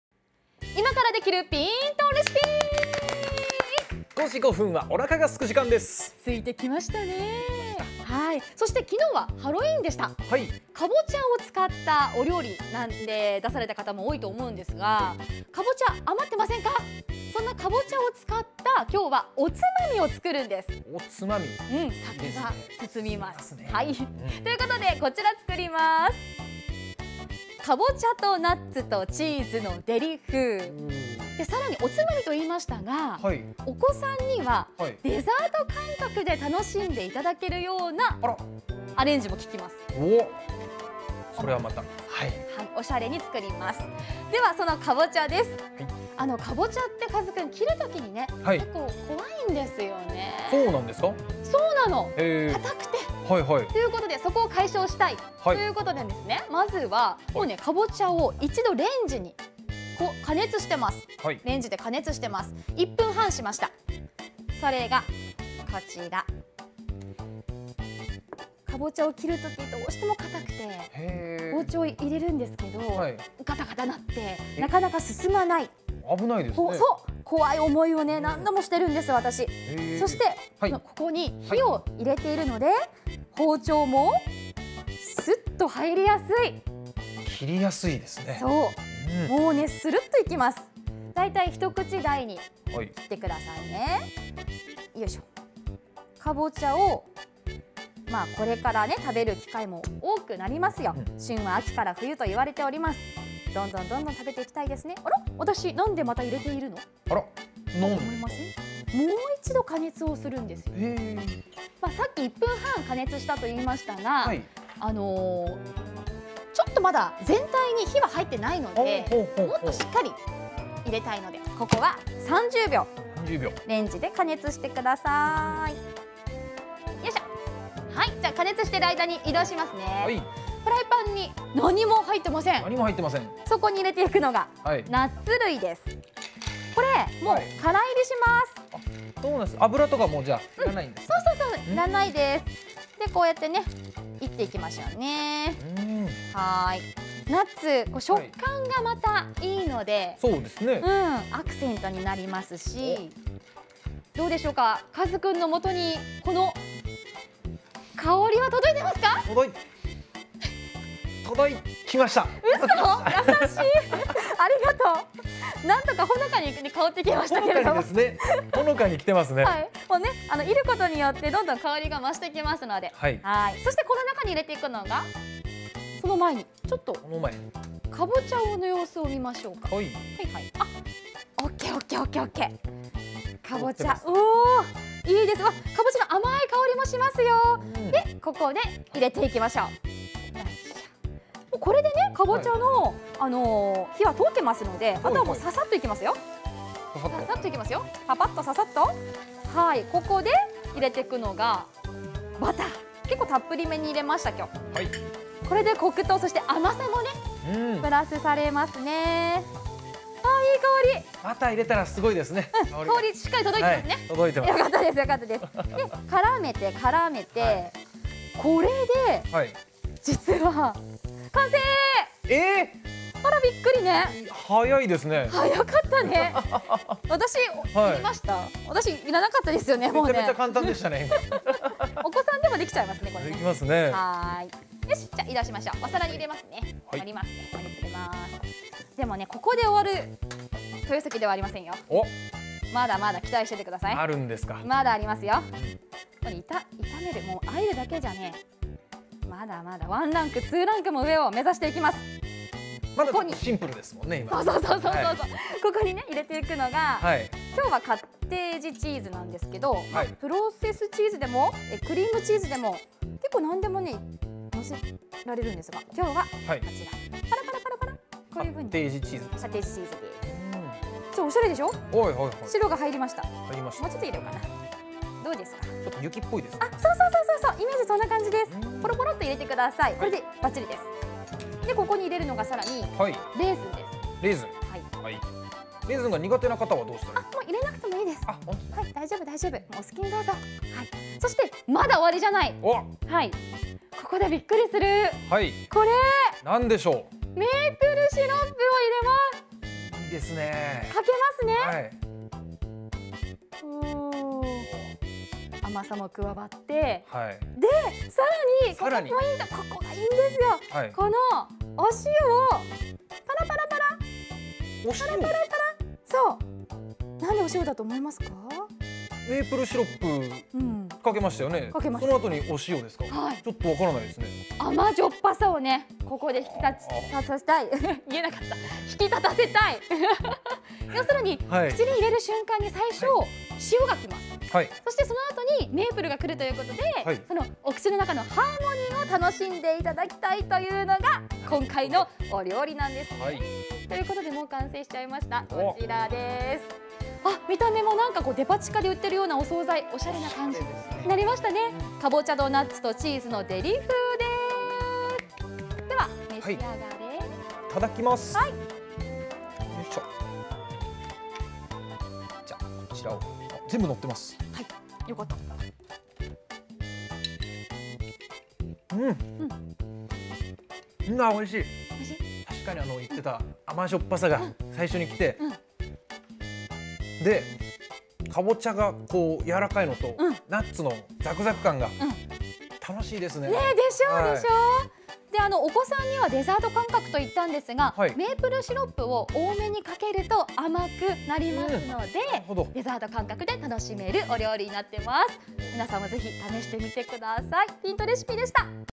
いまからできるPintレシピ「かぼちゃとナッツとチーズのデリ風」
【材料】
かぼちゃ
ナッツ類
クリームチーズ
レーズン
バター
塩
メイプルシロップ

【作り方】
1. かぼちゃは食べやすい大きさに切り、レンジで軽く加熱する。
2. フライパンにナッツを入れて、少し乾煎りする。
3. ②にバターと①入れて炒め、火を止めて器に盛る。
4. クリームチーズとレーズンを散らす。
5. メイプルシロップをかけ、最後に塩を振る。